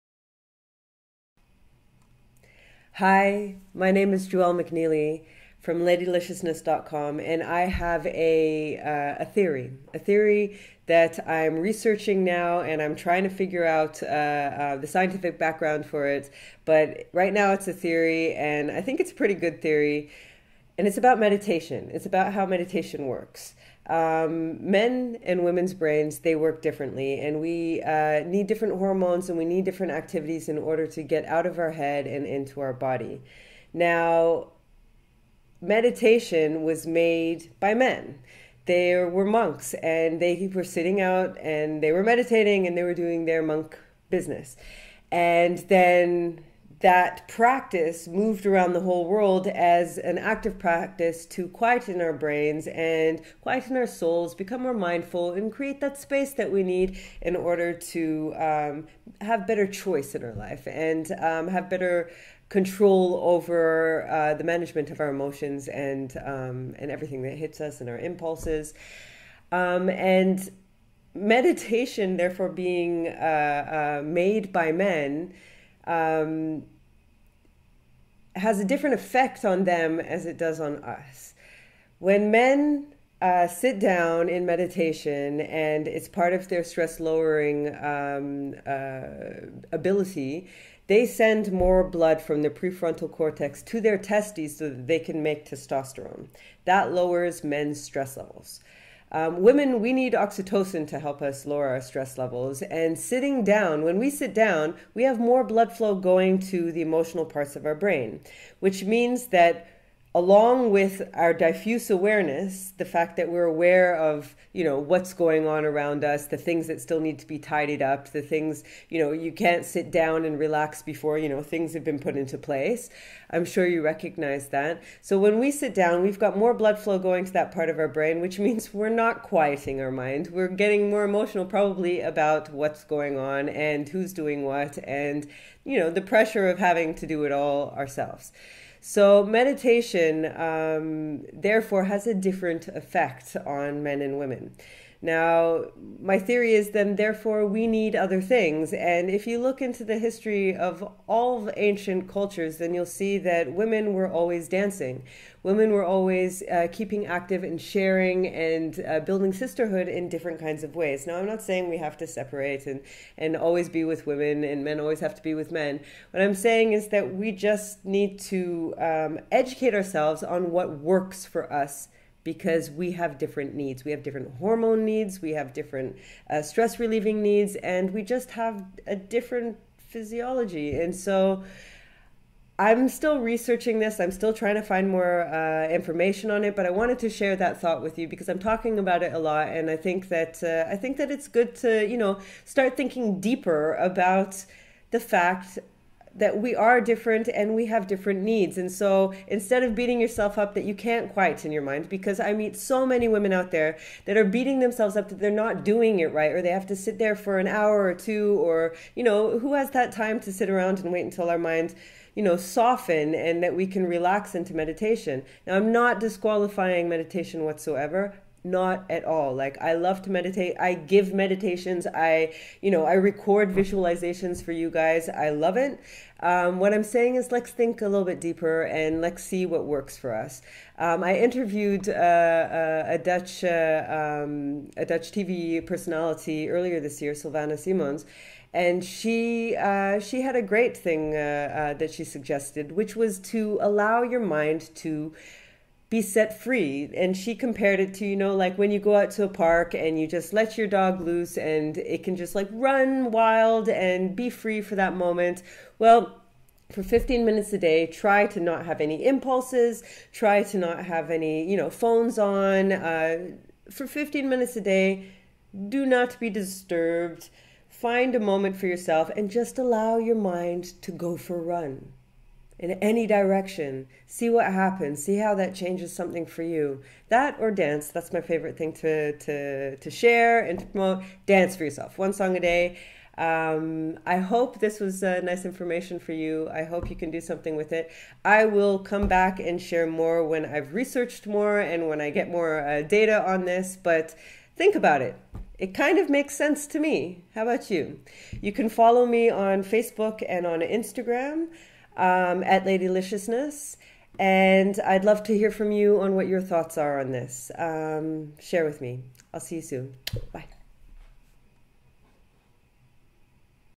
Meditation was made by monks - Juel McNeilly | Ladyliciousness
I have a little theory: meditation works differently on men and women.
We have different brains, hormonal balances, stress relief needs, and stress triggers. Yet we are told that the meditation experience should be the same.

I disagree. I am pro-meditation, but feel the ultimate experience of it differs between women and men. Still researching but let's start thinking more deeply about this, so we can create choice for ourselves.

Hi, I'm Juel McNeilly, and I help dynamic women figure out what their superpowers are, so they can thrive!

Connect and Share with me, Juel⭐